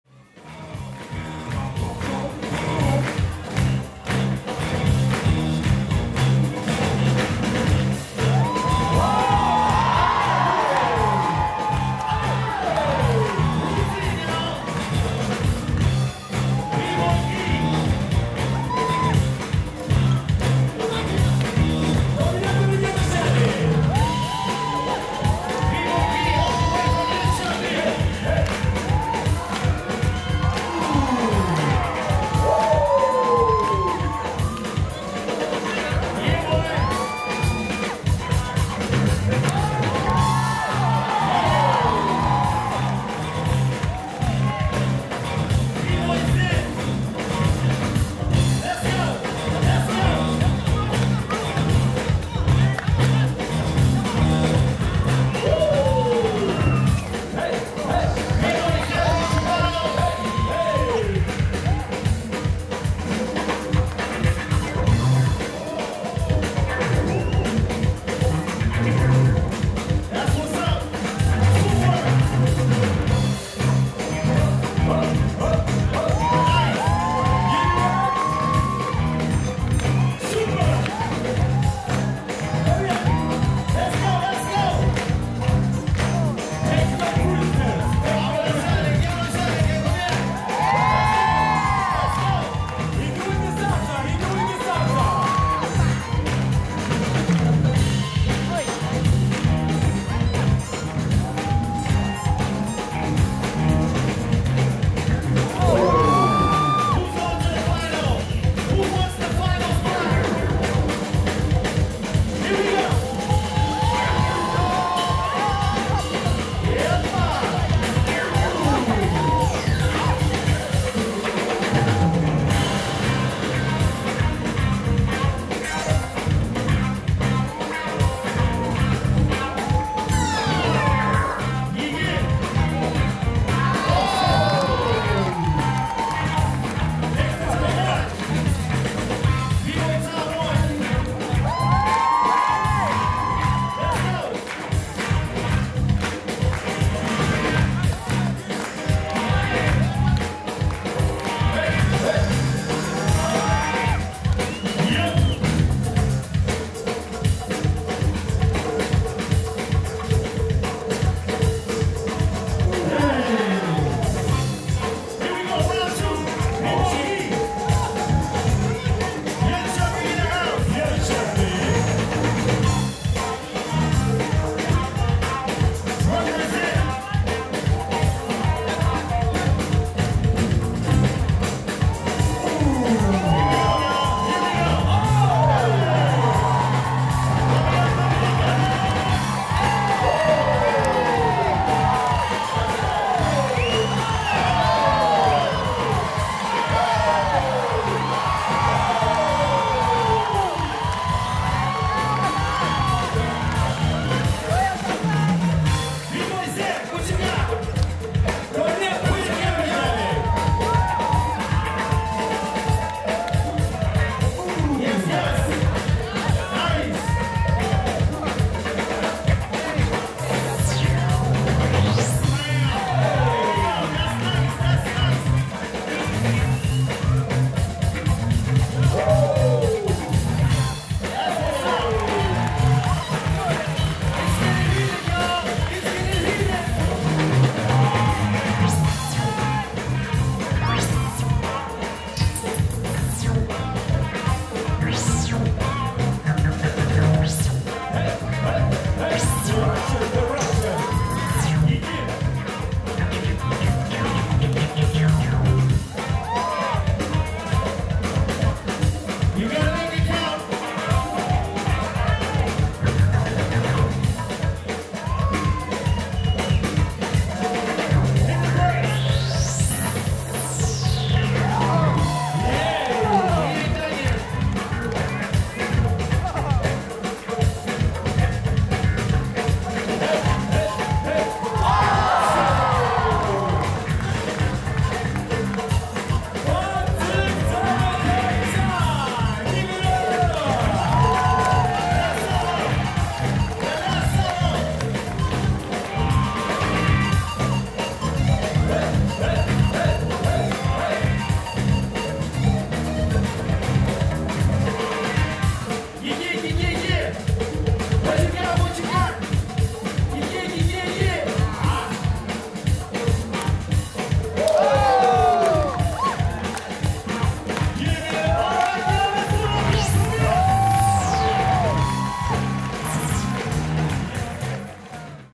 Gbg Dance festival 2015- Semifinal Break BROTHER BREAKERS VS BBOY TOM ONE & BBOY ZEN
GBG Dance Festival 2015 - Gbg city Jam
The festival is part of a dance plattform project
called Community X - a non profit youth project by Twistedfeet Organisation.
The festival includes workshop camp, showcases, battles & jam.
Supported by Arvsfonden, Dance Culture Df, Bilda, Kultur Ungdom, Kultur i Väst,Göteborgs Kulturförvaltning, Göteborg & Co